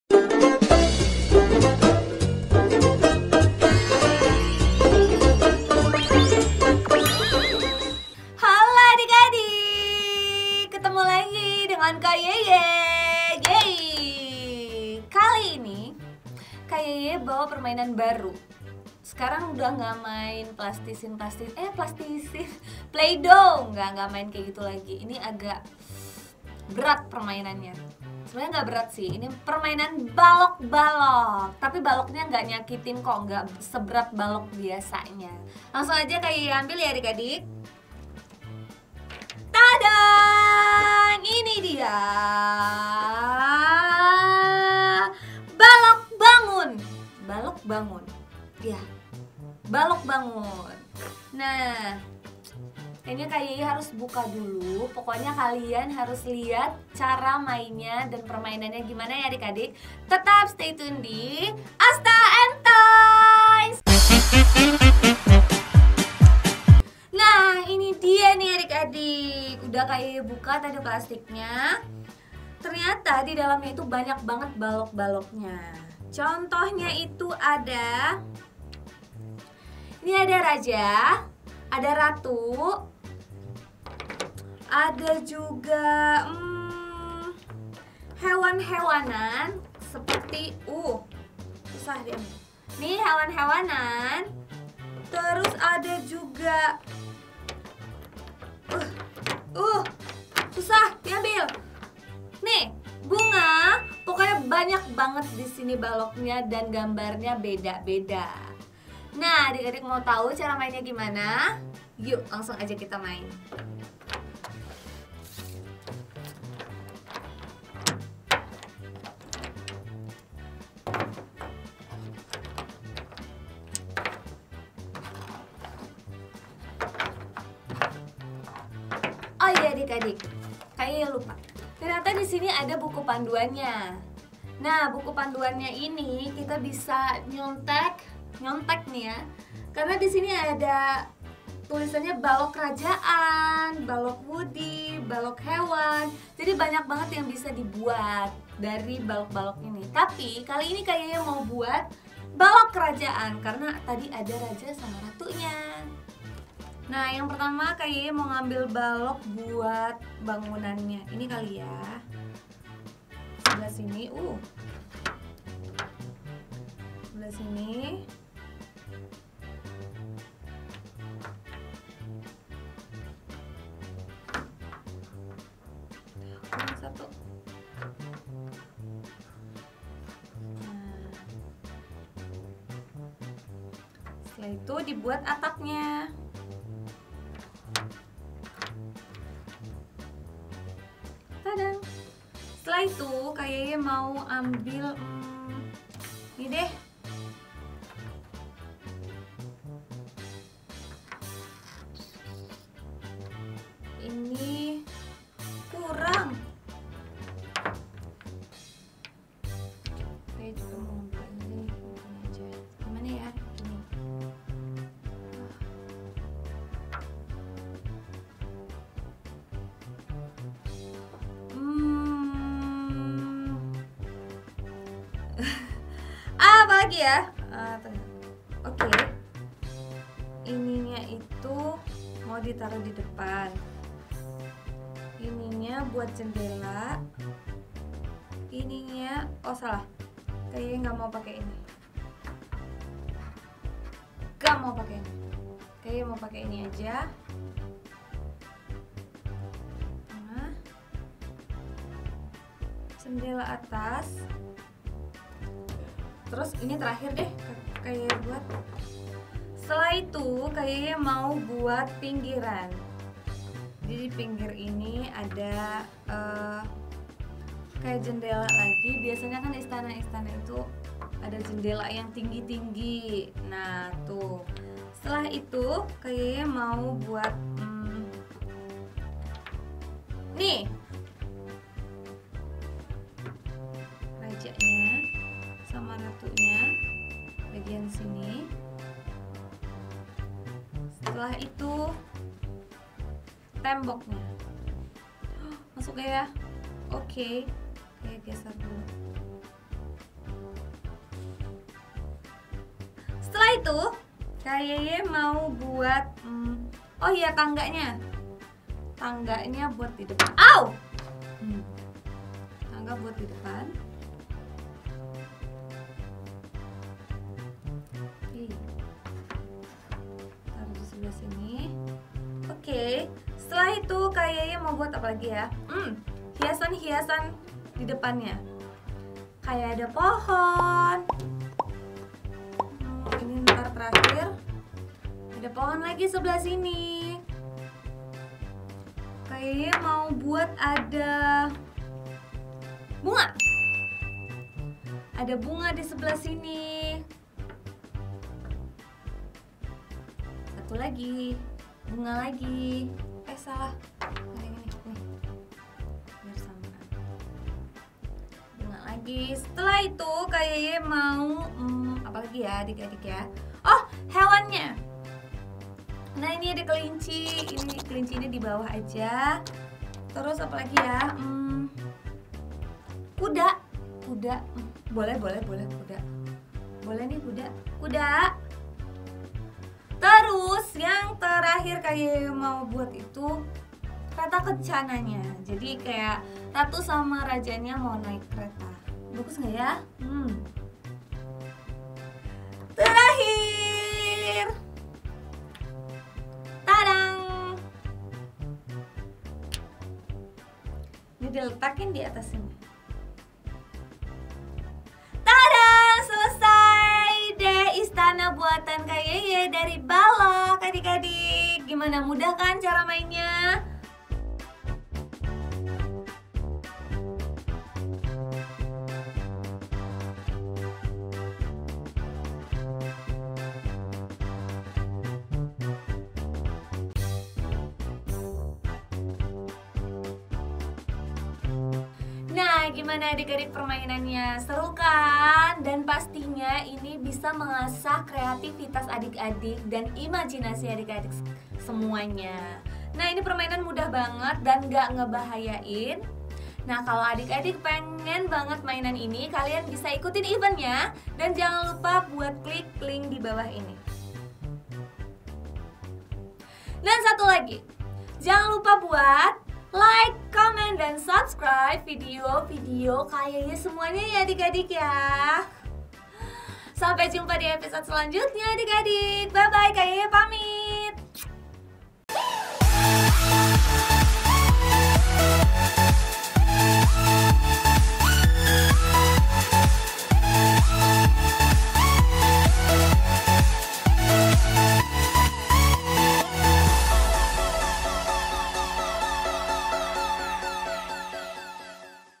Balok Istana Kayu buatan Ka Yeye - Asta And Toys
Ka Yeye ingin Membuat Istana seperti apa ya dengan mainan Baloknya? Penasaran? Coba klik videonya deh^^

Nah di Fanpage Asta And Kidz sedang bagi-bagi mainan gratis lho, kalian pasti mau dong mainan gratis? Yuk klik Link dibawah ini!